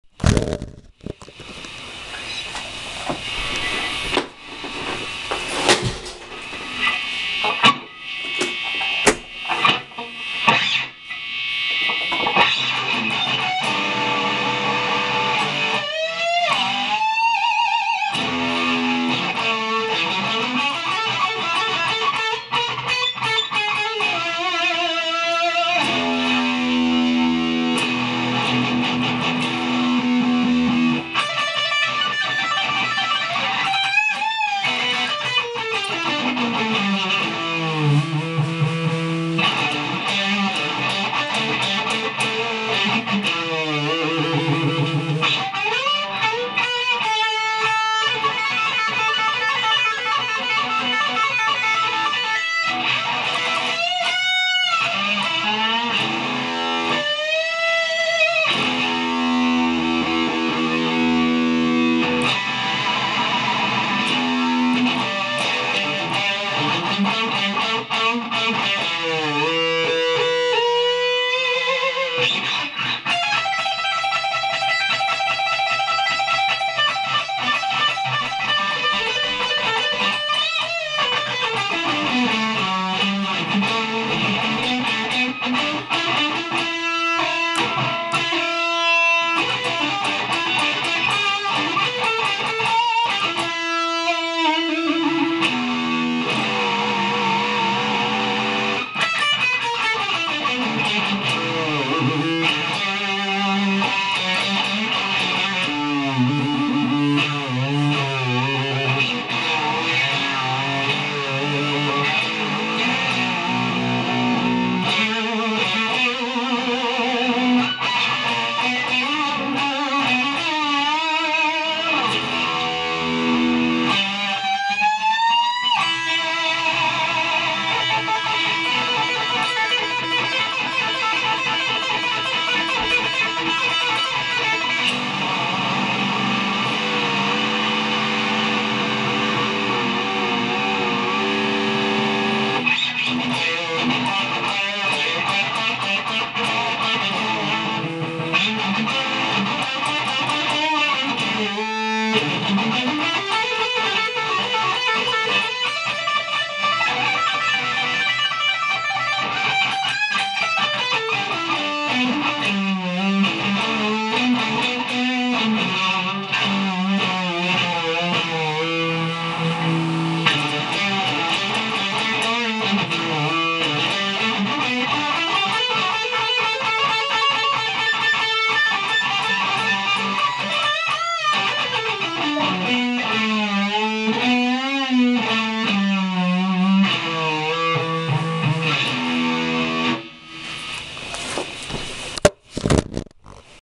DR Neon Green Guitar Strings Test
Trying out some DR Neon guitar strings. This was filmed under a blacklight, sorry about grainy image, I had to turn the light receiver thing all the way up to get the glow of the strings to show up on camera.